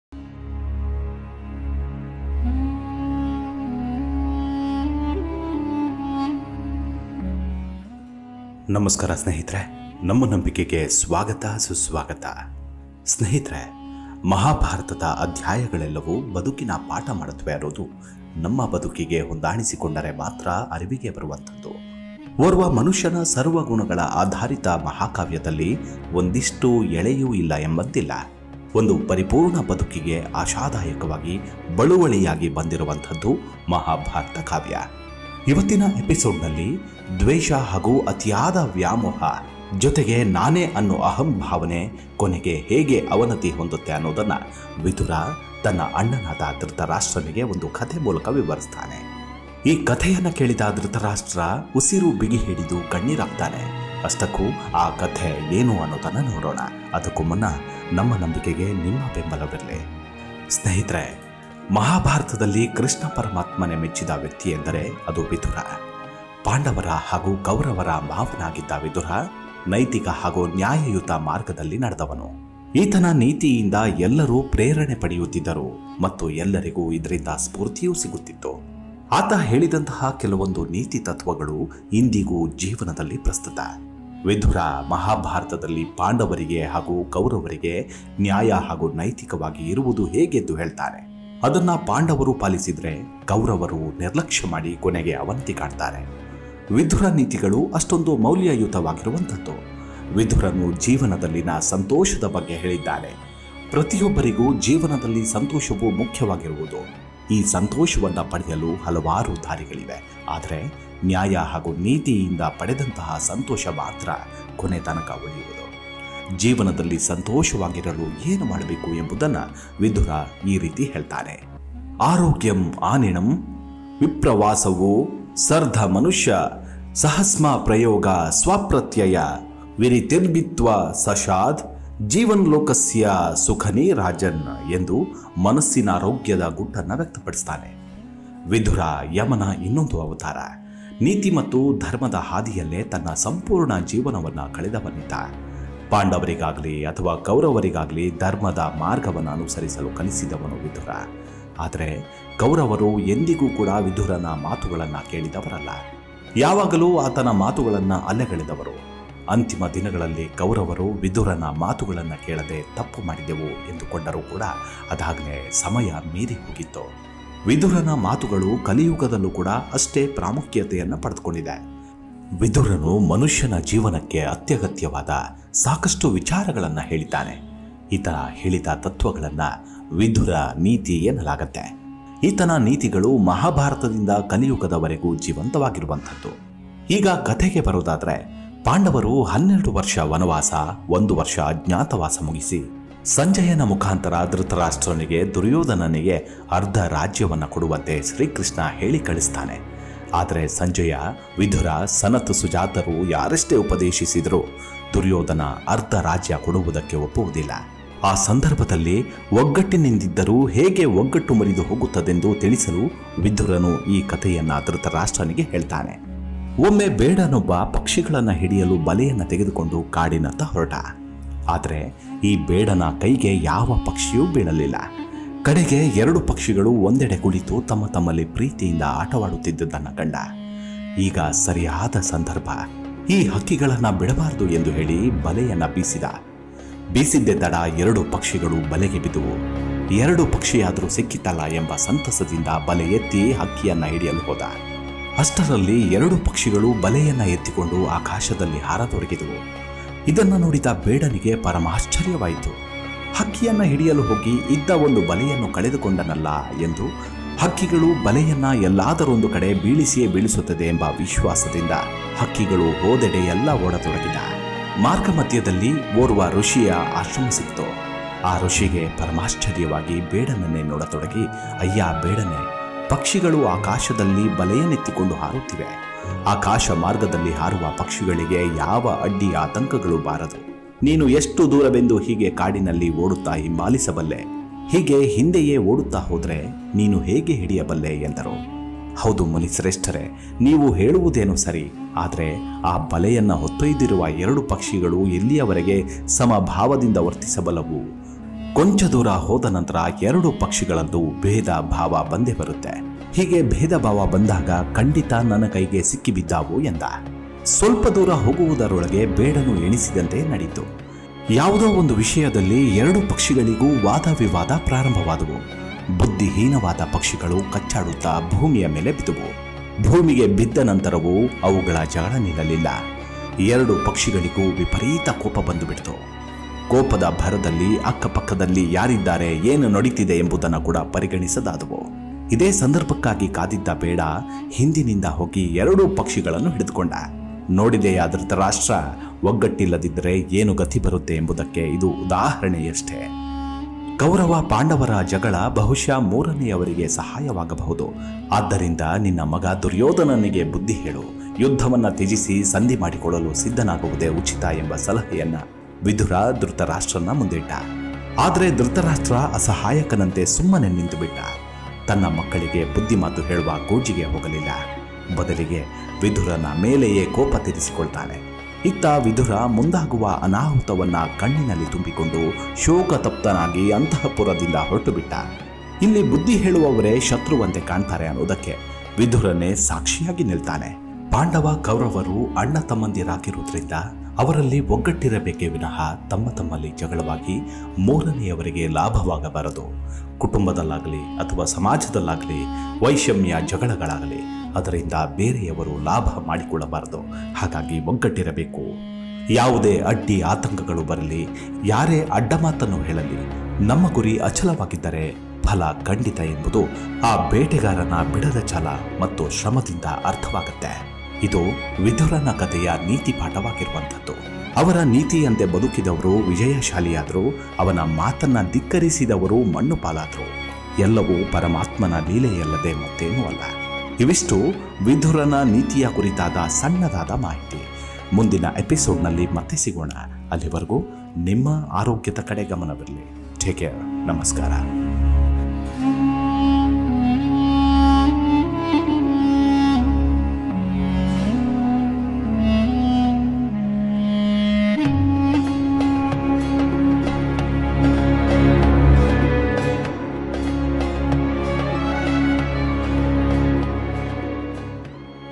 ವಿದುರ ಹೇಳಿದ ಕಥೆ ಕೇಳಿ ಧೃತರಾಷ್ಟ್ರ ಇನ್ನಿಲ್ಲದಂತೆ ಯಾತನೆಪಟ್ಟಿದ್ದ | The moral story of vidura
@nammanambike spirituality spiritual kannadamotivationalvideo kannada youtube video